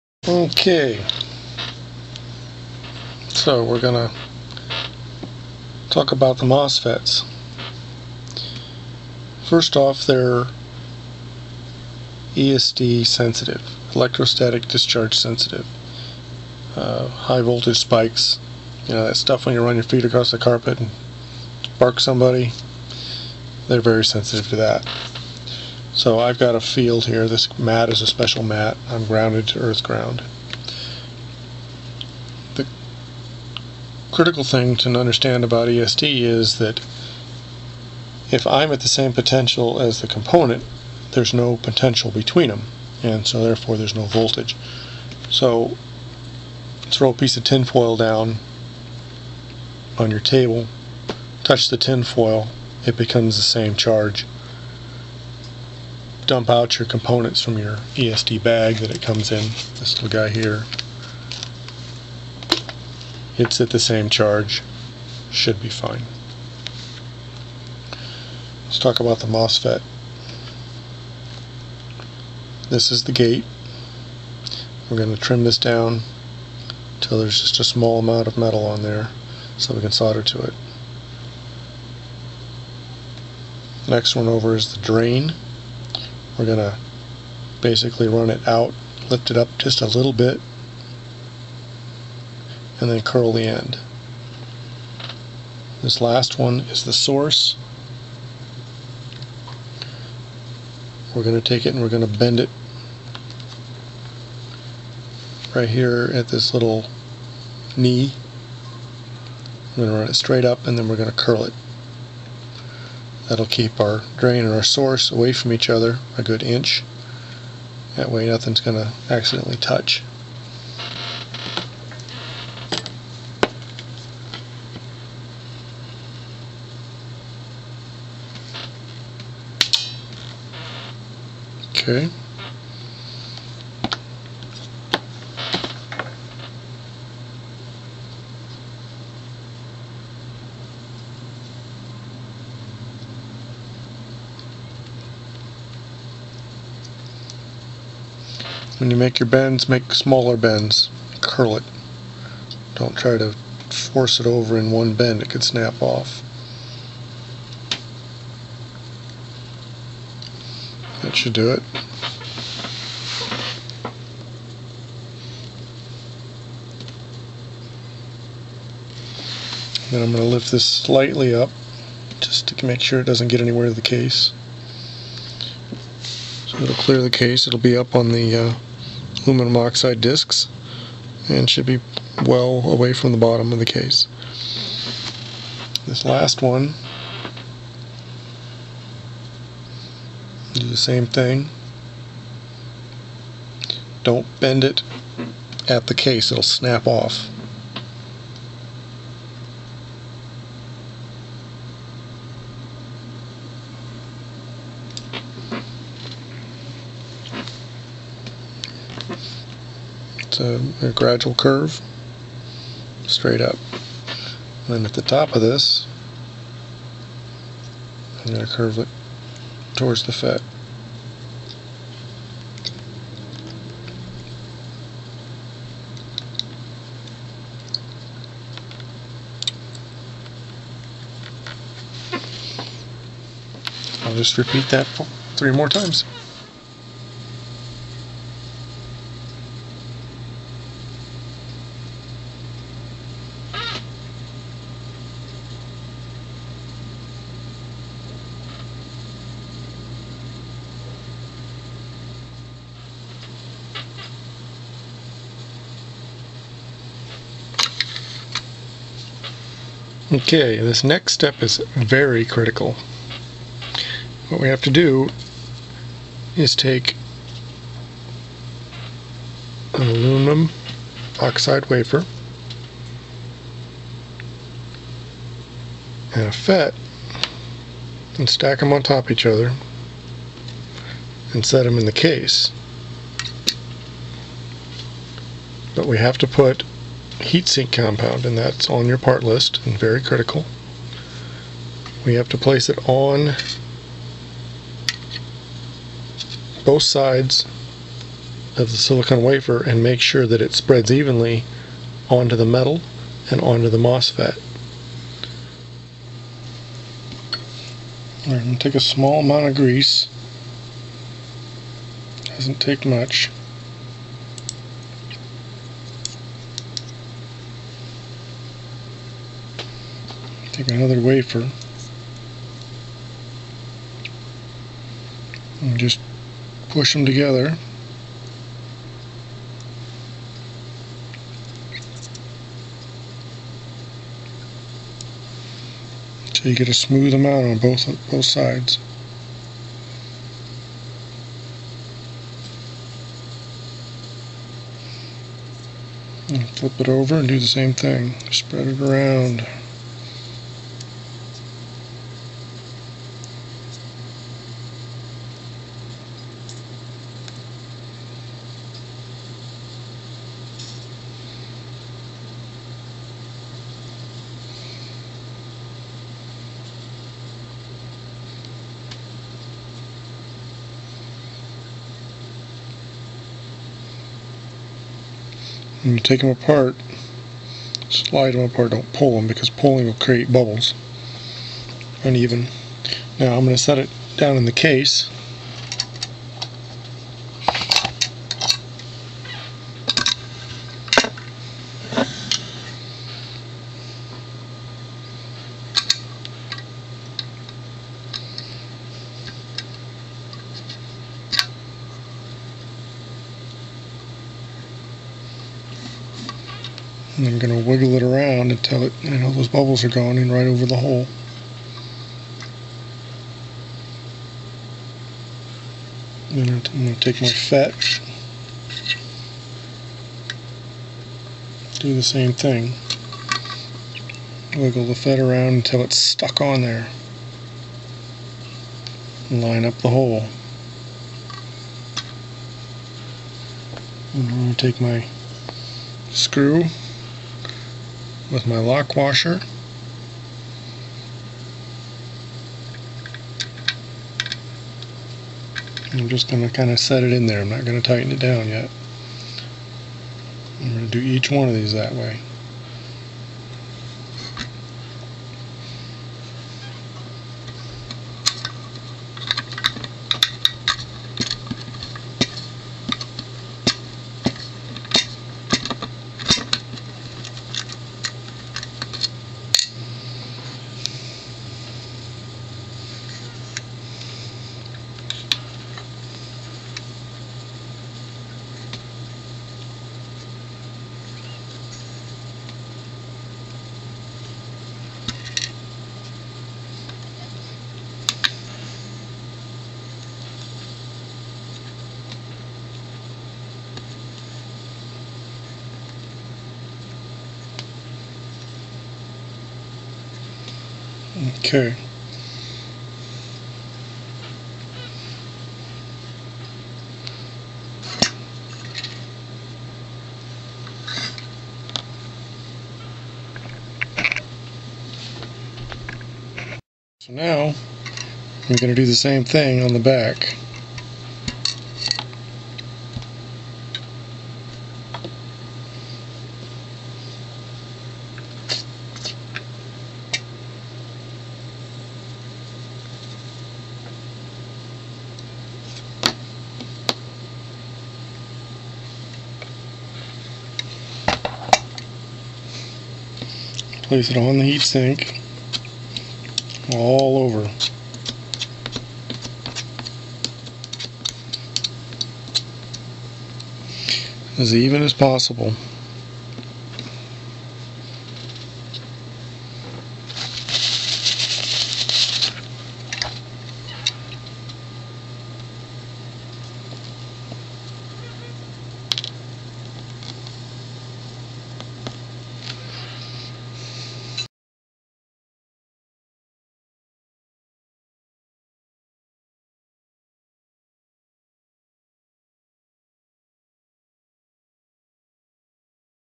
#27 H2O2 - PWM Case V2 Layout Part 5 of 17
In this episode MosFet Electro Static Discharge (ESD) Handling, Bending MosFet Pins, Building the Heat Sink System. Applying Heat Sink Compound.
This is My Concept of what a PWM case should look like. Minimal cooling, no fan! Why, Because The Four MosFets are not driven hard. No moving parts to fail.  Automatic Current Control! Why, Because without it any HHO cell will heat up and reach thermal runaway: Current increase and the cell gets hotter this continues until the cell melts or the current source fails or the fuse blows. A Function to monitor  Peek Current! Why, because the System will fail if the peek current is to high. Knowing peek current means you are in control of your Cell and its Electrolyte.